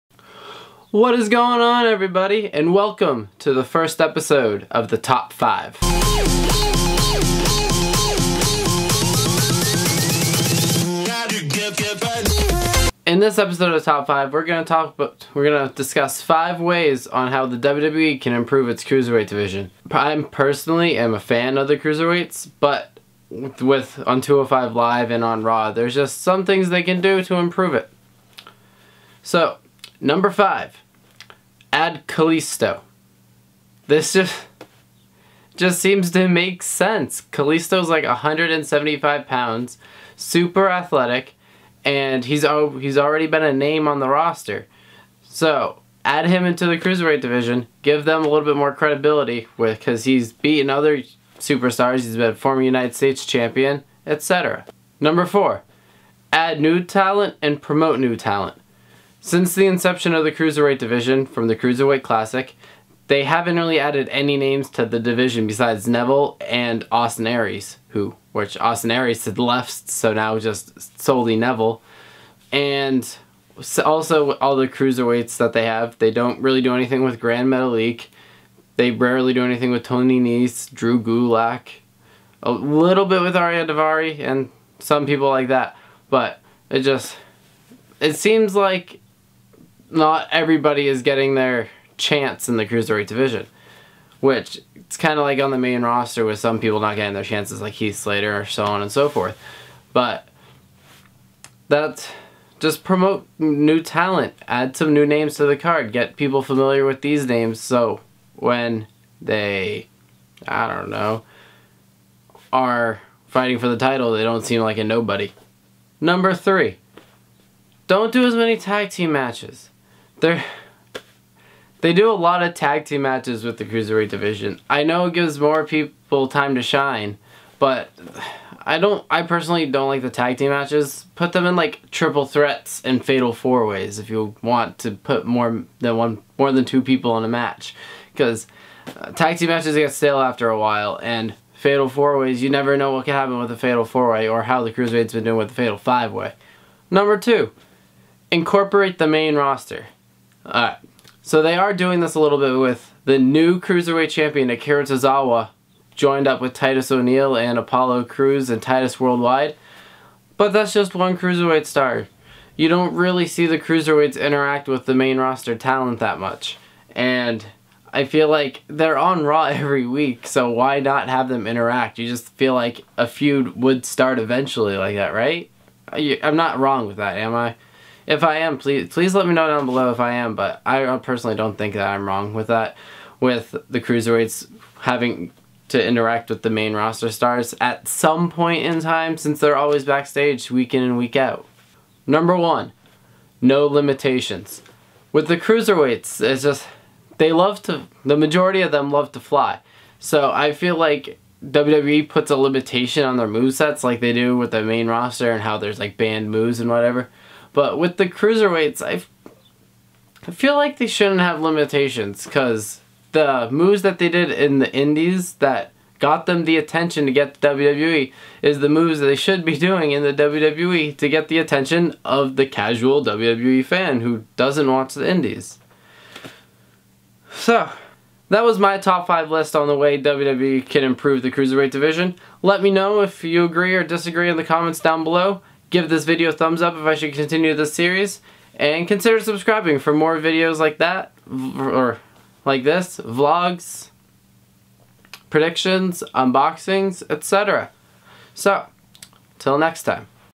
5 Ways WWE Can Improve The Cruiserweight Division | The Top 5
The WWE Cruiserweight Division has been off to a rocky start, here are some ways they can improve it.